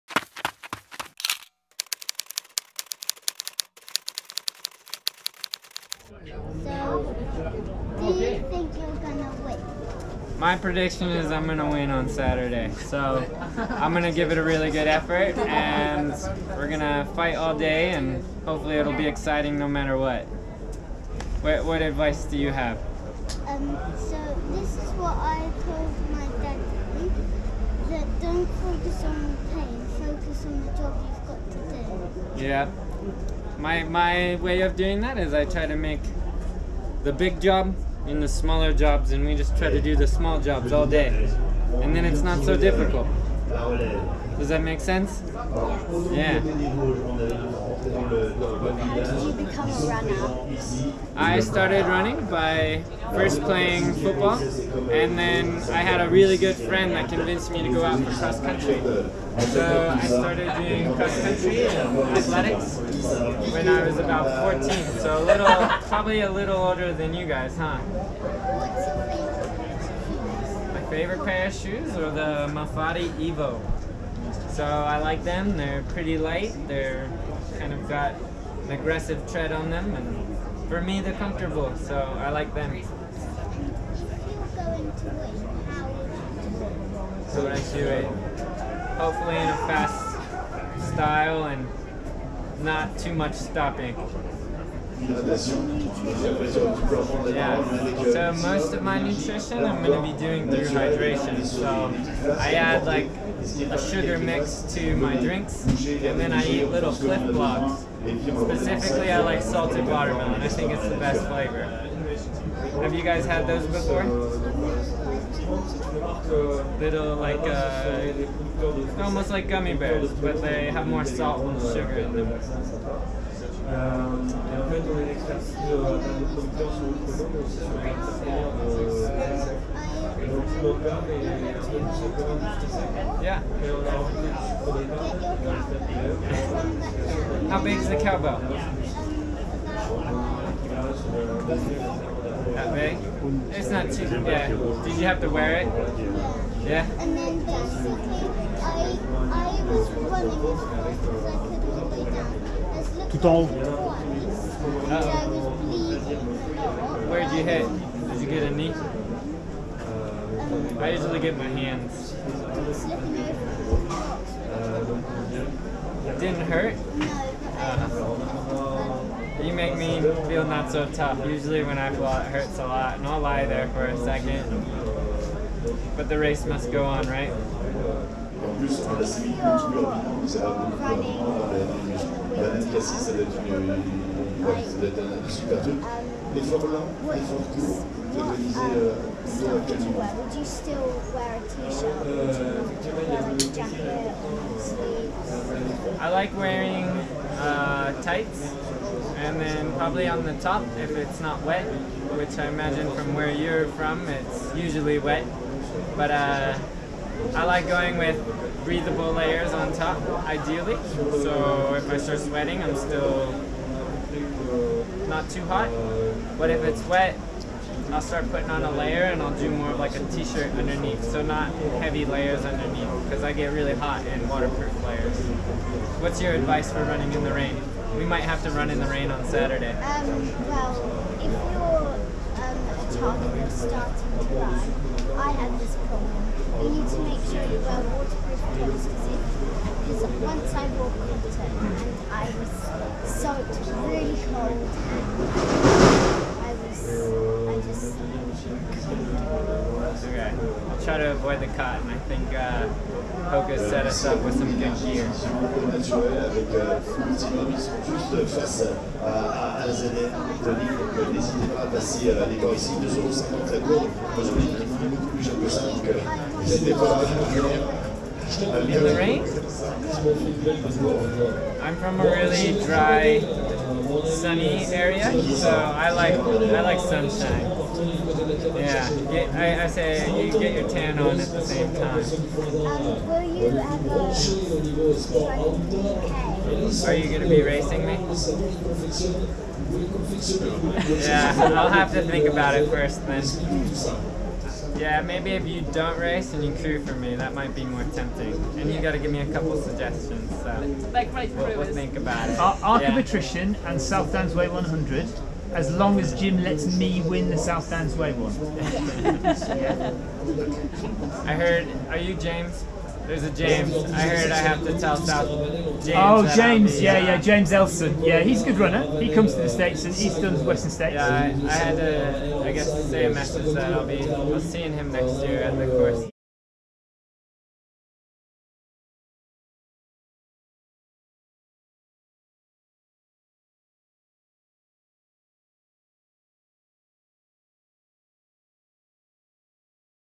Jim Walmsley Interviewed by my kids | UTMB 2018
Jim Walmsley is probably the world's best ultra runner. Two days prior to the 2018 UTMB, my children were given the opportunity to interview Jim, winner of the 2018 Western States 100 and arguably the only person capable of beating Kilian Jornet.

If you would like me to film your run, whether you are taking part in or organising an event, do get in touch